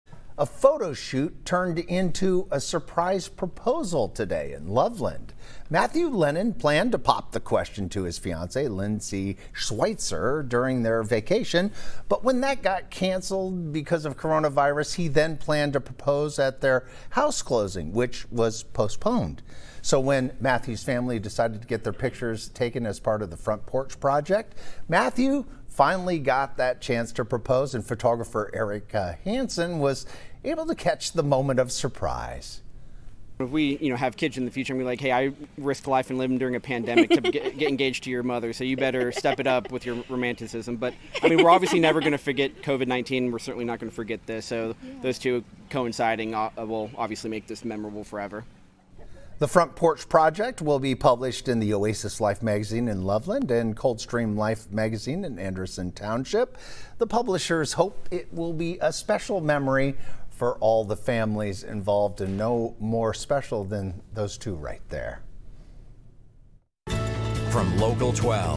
Magazine photo shoot turns into surprise proposal
LOVELAND, Ohio (WKRC) - A photo shoot turned into a surprise proposal Sunday in Loveland.

Matthew Lennon planned to pop the question to his fiance, Lindsay Schweitzer, during their vacation.

When that got cancelled because of the coronavirus, he planned to propose at their house closing, which was also postponed.

So, when Matthew's family decided to get their pictures taken as part of the Front Porch Project, Matthew finally got to propose.

Photographer Erica Hansen was able to catch the moment of surprise.

"If we have kids in the future I'm going to be like hey, I risked life and limb during a pandemic to get engagement to your mother so you better step it up with your romanticisim, but we're obviously never going to forget COVID-19 and we're not going to forget this so those two coinciding will make this memorable forever," said Lennon.

The Front Porch Project will be published in the Oasis Life Magazine in Loveland and Coldstream Life Magazine in Anderson Township.

The publishers hope it will be a special memory for the families involved.